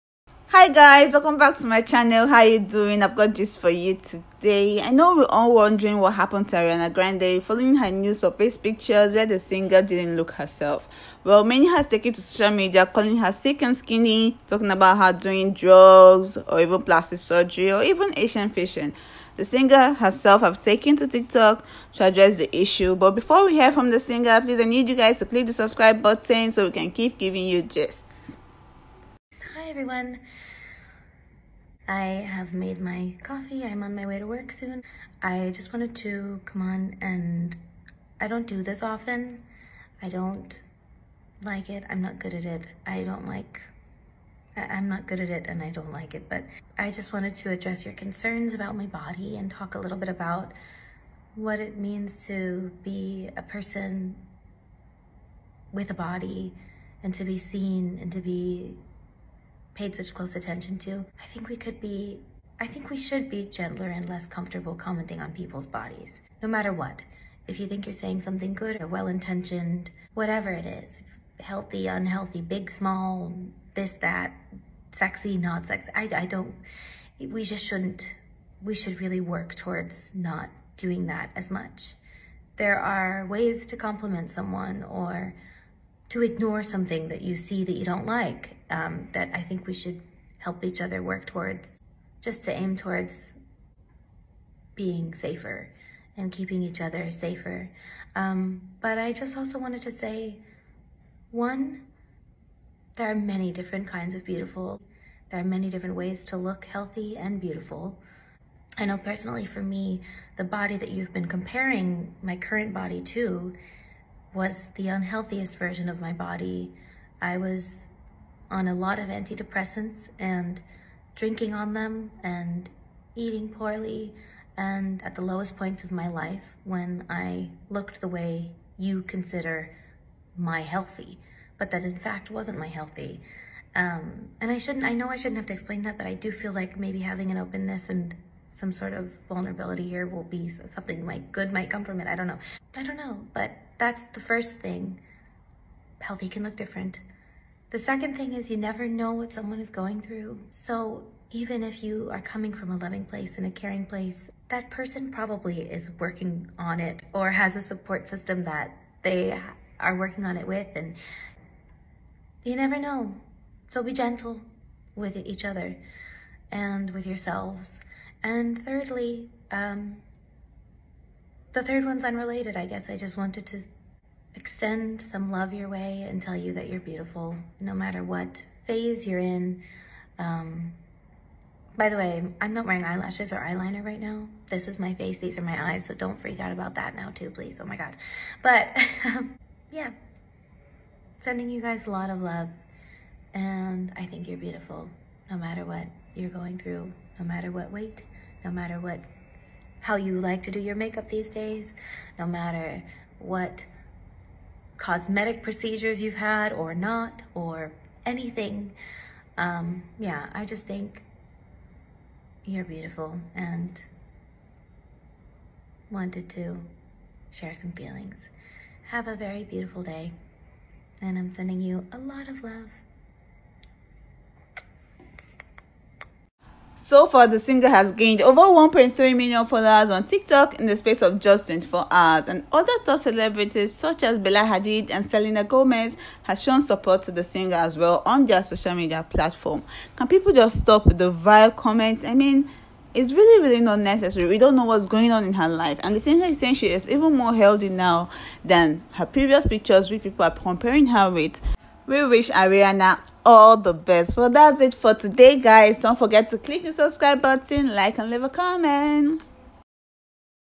WHAT HAPPENED TO ARIANA GRANDE
Concerns has been drawn towards singer ARIANA GRANDE recent looks. The singer has taken to her TikTok to address the rising issue.

Is Ariana Grande sick
Ariana Grande new look
Ariana Grande TikTok
Ariana Grande new video